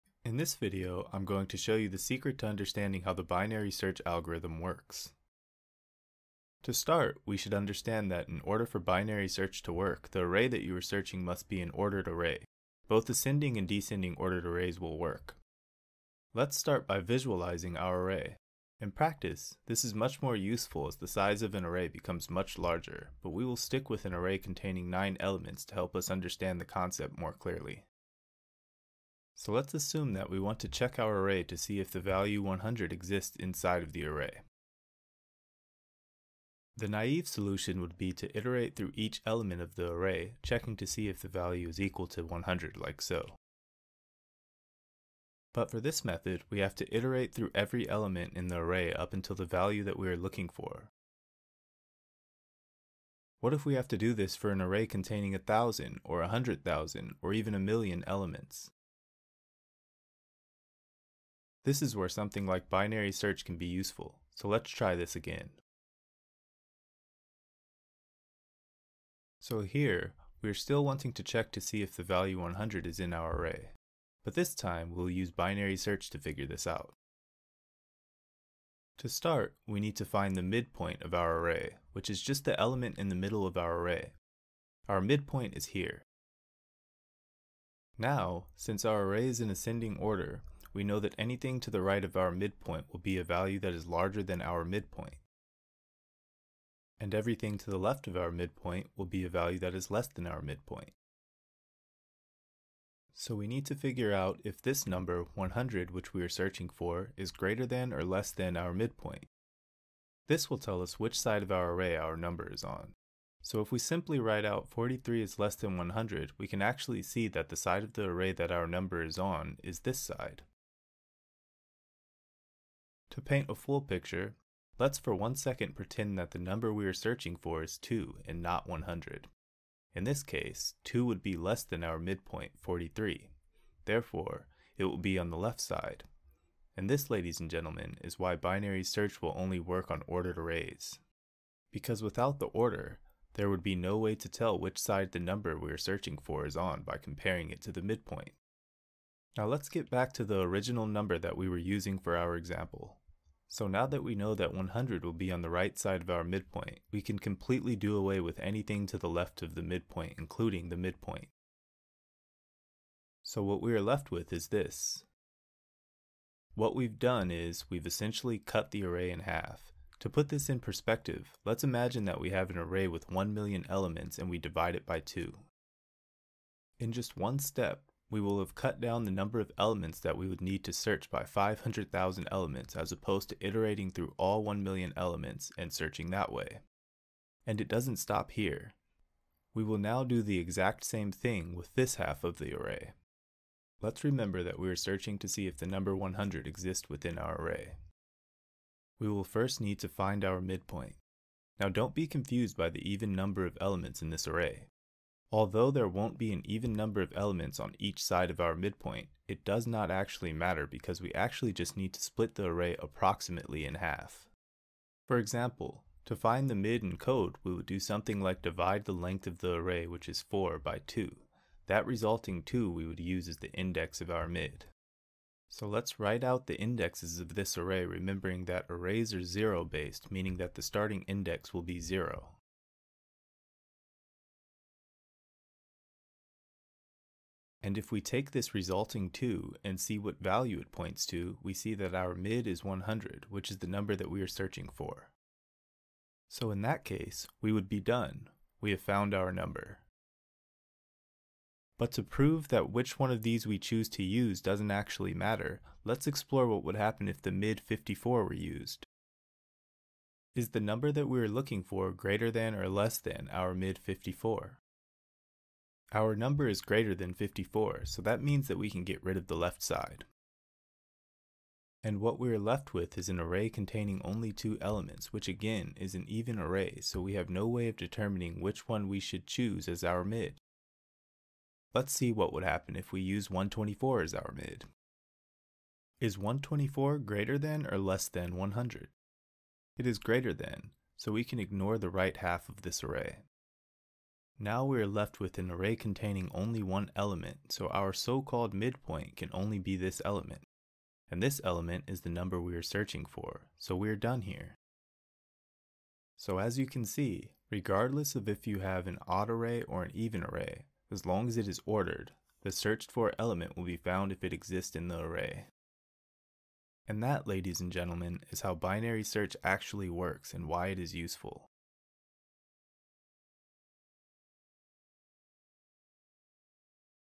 Learn Binary Search Fast
In this video you will learn binary search fast. You will learn the basics of the binary search algorithm in 5 minutes.